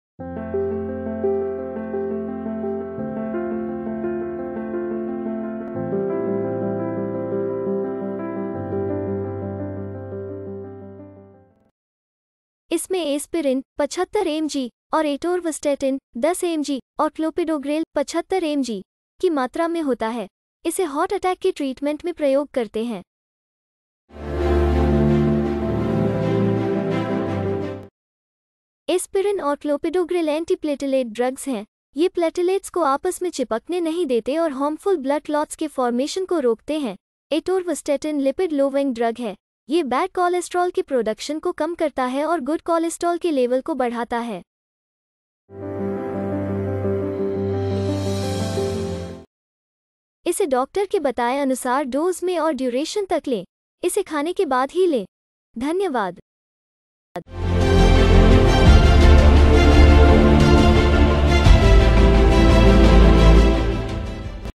Clavix Gold 10 Capsule in Hindi
" Know more about Clavix Gold 10 Capsule at  . View medicine information in Hindi.
Introduction
Clavix Gold 10 Capsule should be taken with food regularly at a fixed time each day. You should take it regularly at evenly spaced intervals as per the schedule prescribed by your doctor. Taking it at the same time every day will help you remember to take it. The dose and duration of treatment will depend on what you are being treated for. Do not stop taking it until you have finished the complete course, even when you feel better. Lifestyle changes like low-fat diet, exercise and not smoking may help this medicine to work better.इस दवा को लेने से पहले, अपने डॉक्टर को बताएं कि क्या आपको लीवर या किडनी की कोई बीमारी है। यदि आप गर्भवती हैं, गर्भावस्था की योजना बना रही हैं या स्तनपान करा रही हैं तो आपको अपने डॉक्टर को अवश्य बताना चाहिए। आपको अपनी स्वास्थ्य सेवा टीम को अन्य सभी दवाओं के बारे में भी बताना चाहिए जो आप ले रहे हैं क्योंकि वे इस दवा से प्रभावित हो सकती हैं या प्रभावित हो सकती हैं।
इस दवा को अपने चिकित्सक द्वारा बताई गई खुराक और अवधि में लें। क्लैविक्स गोल्ड 10 कैप्सूल को भोजन के साथ लेना बेहतर होता है.
क्लैविक्स गोल्ड 10 कैप्सूल तीन दवाओं का एक मिश्रण हैःएस्पिरिन, एटोरवास्टेटिन और क्लोपिडोग्रेल, जो दिल के दौरे से बचाता है. एस्पिरिन और क्लोपिडोग्रेल एंटीप्लेटलेट दवाएं हैं। वे प्लेटलेट्स को आपस में चिपके रहने से रोकते हैं और हानिकारक रक्त के थक्कों को बनने से रोकते हैं। एटोरवास्टेटिन एक लिपिड-कम करने वाली दवा है जो शरीर में कोलेस्ट्रॉल बनाने के लिए आवश्यक एंजाइम को अवरुद्ध करती है। यह ""खराब"" कोलेस्ट्रॉल (एलडीएल), ट्राइग्लिसराइड्स को कम करता है और ""अच्छा"" कोलेस्ट्रॉल (एचडीएल) बढ़ाता है।"